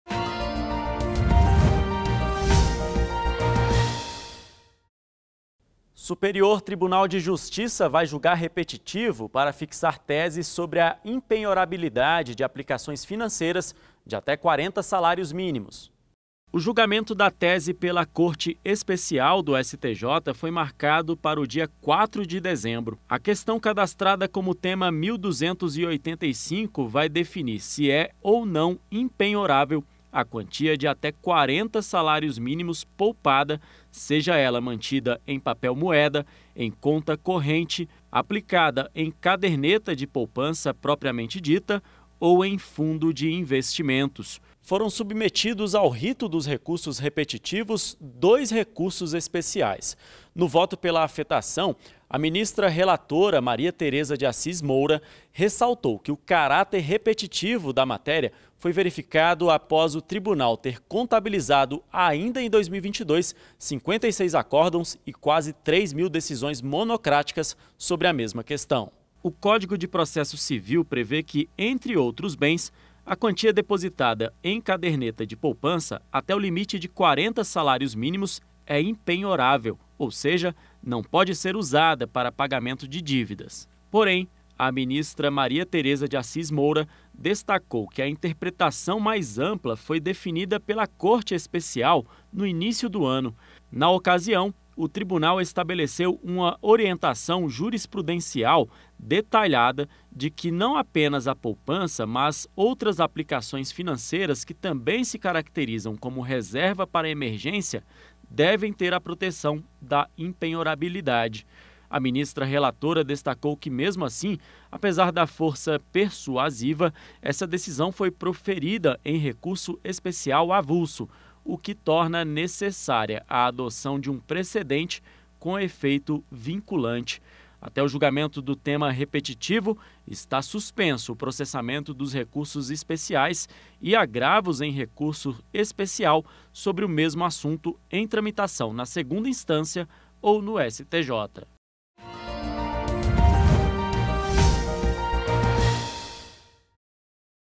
Repetitivo fixará tese sobre impenhorabilidade de aplicações financeiras até 40 salários mínimos
​A Corte Especial do Superior Tribunal de Justiça (STJ) deve julgar no dia 4 de dezembro, sob o rito dos repetitivos, os Recursos Especiais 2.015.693 e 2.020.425, de relatoria da ministra Maria Thereza de Assis Moura.
A questão submetida a julgamento, cadastrada como Tema 1.285 na base de dados do STJ, é "definir se é ou não impenhorável a quantia de até 40 salários mínimos poupada, seja ela mantida em papel-moeda, em conta-corrente, aplicada em caderneta de poupança propriamente dita ou em fundo de investimentos".
REsp 2015693
REsp 2020425